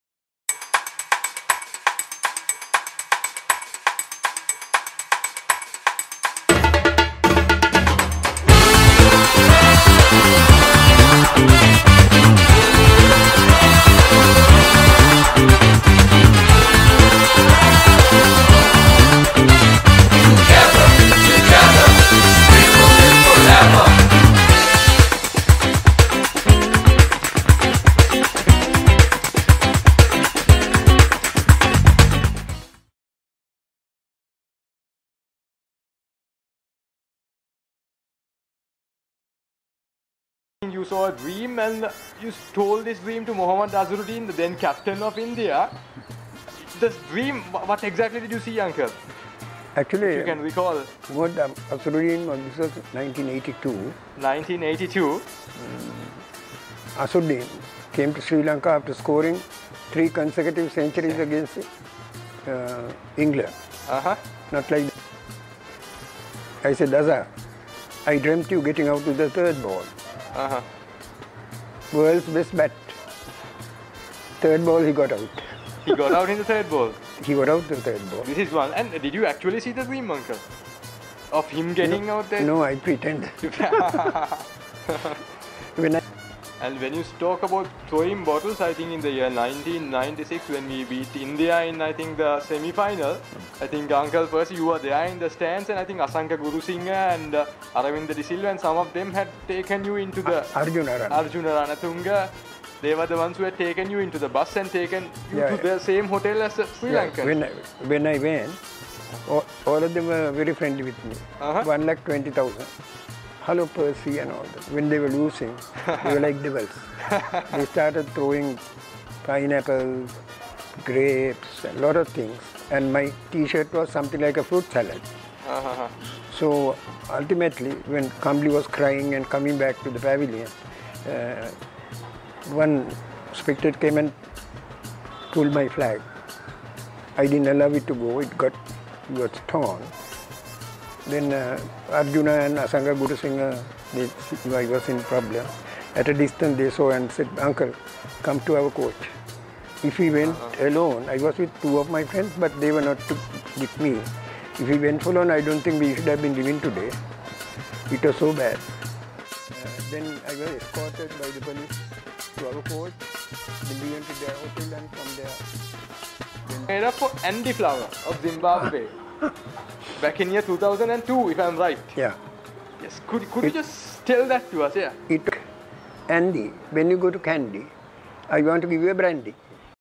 I predicted Dimuth's captaincy, even before he dreamt of it - Uncle Percy (TRAILER)
A little tribute to the one and only Percy Abeysekara (Uncle Percy)
for everything, you have to done to raise the spirit of Sri Lanka Cricket!
I am indeed honoured to meet and write about him... The interview will be going on as episodes on "the Sunday Morning."
Hope you enjoyed the trailer. The complete video will be on my Youtube channel soon.
Cheers.